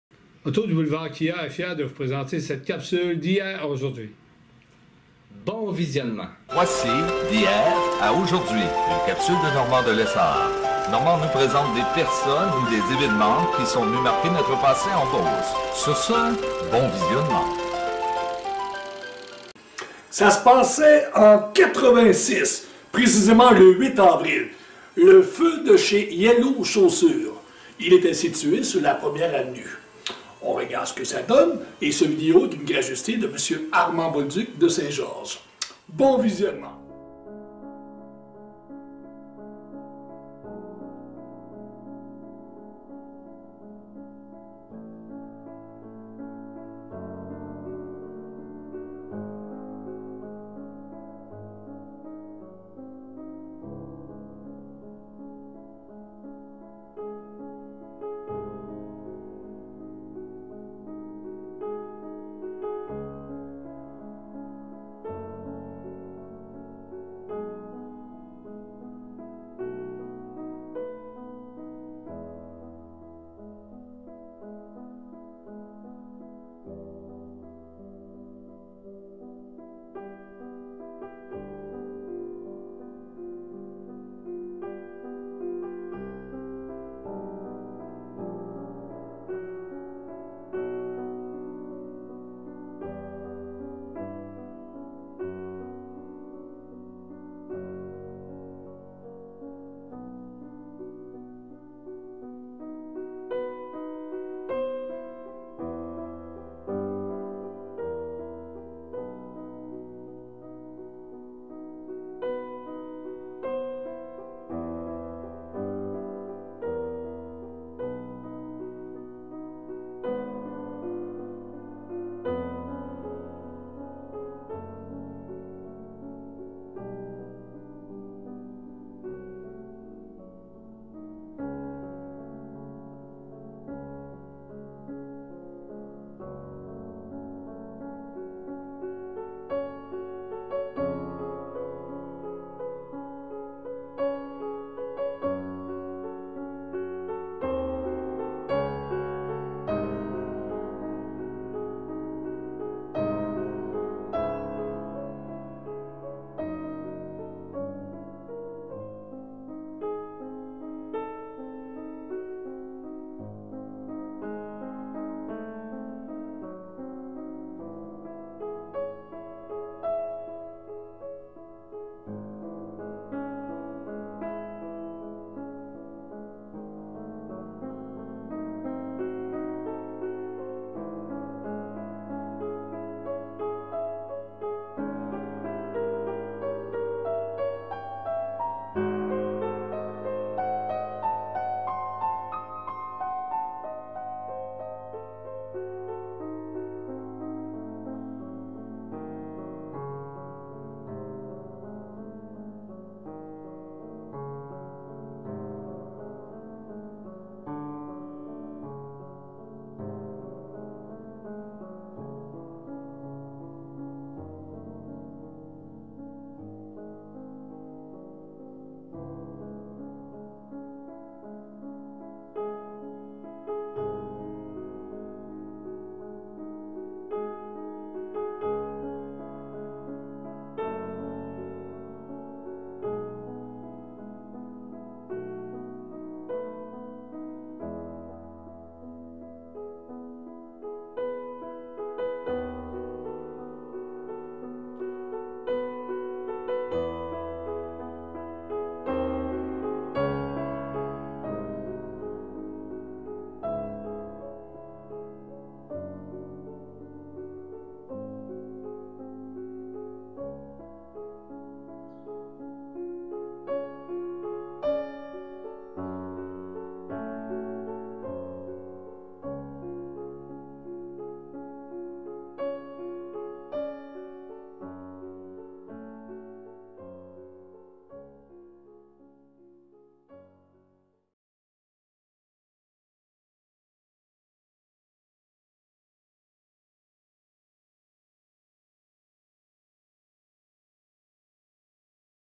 Feu de Yellow Chaussures en 1986(Hier@Aujourd`hui)
Normand présente l`incendie du commerce Yellow chaussures de St-Georges de Beauce en 1986.